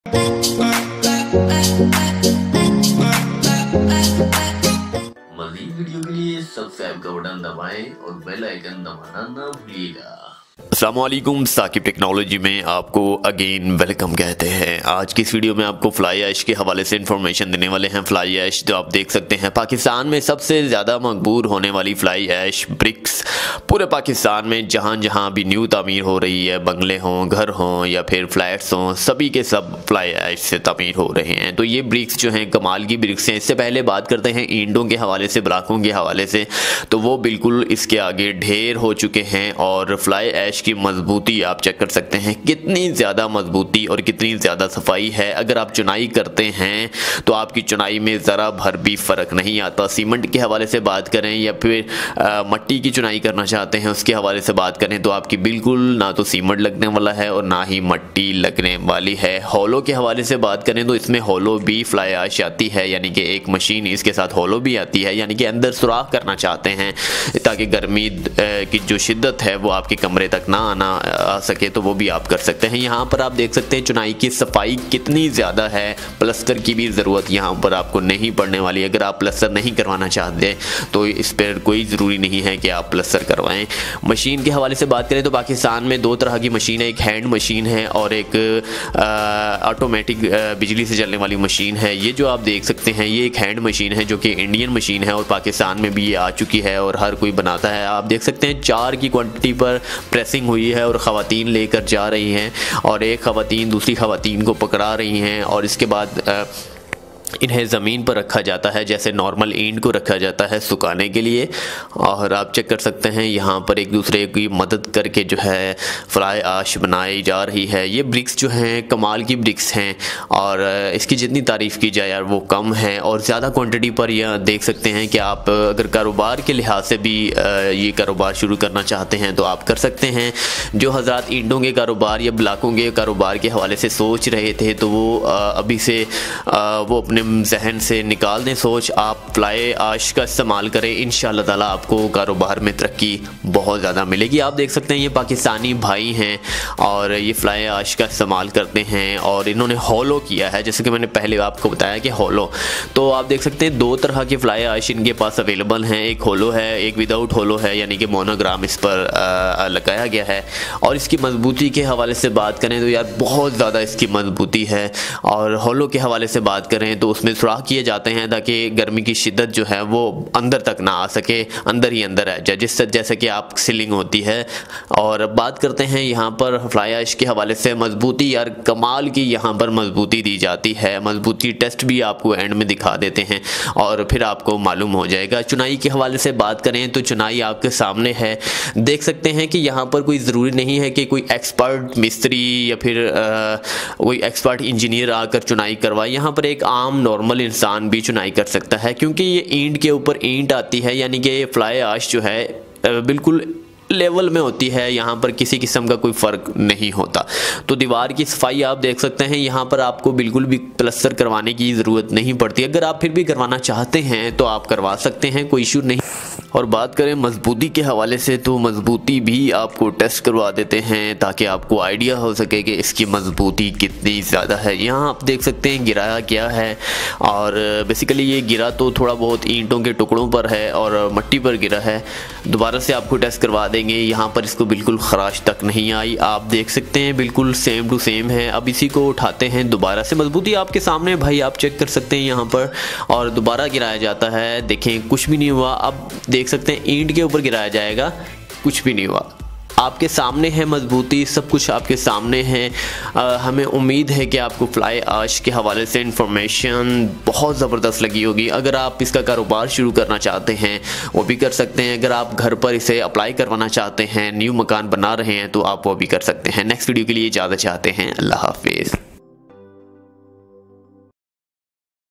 Fly Ash Bricks Business And Home Construction New Technology Materials in Pakistan
Freind Boht Zabardast Technology Hai isi se apne makan banwahein shukriya